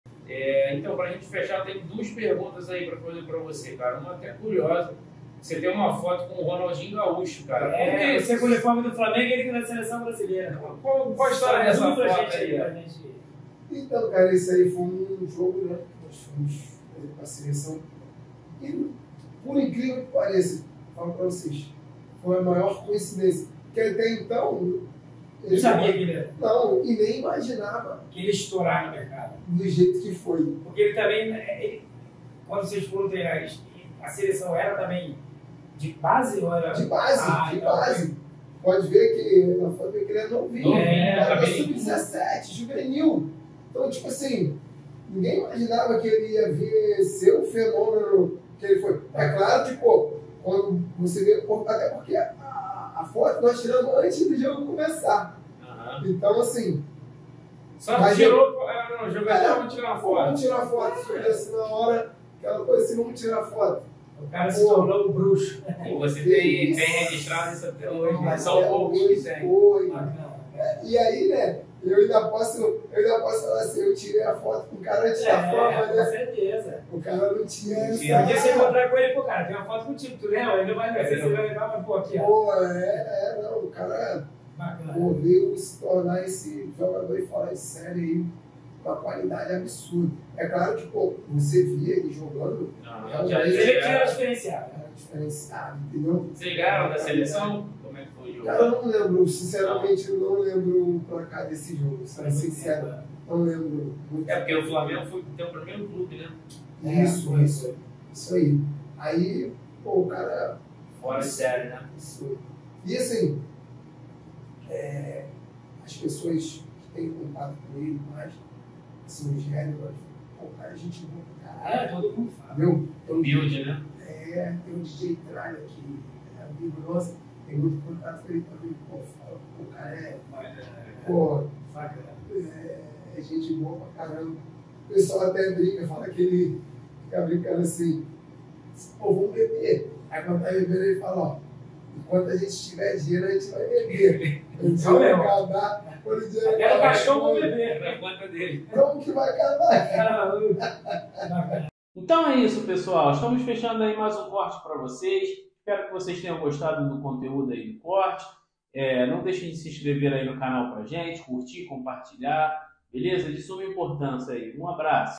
MARCELINHO CONTA COMO FOI JOGAR COM RONALDINHO GAUCHO ANTES DA FAMA ⚽️🔥
Flamengo disputou um amistoso contra a seleção brasileira sub-17 na época em que Ronaldinho Gaúcho era convocado.

Somos o dribla aí, canal interativo e que te convida a conhecer o mundo do futebol e o que é relacionado a ele de uma forma diferente.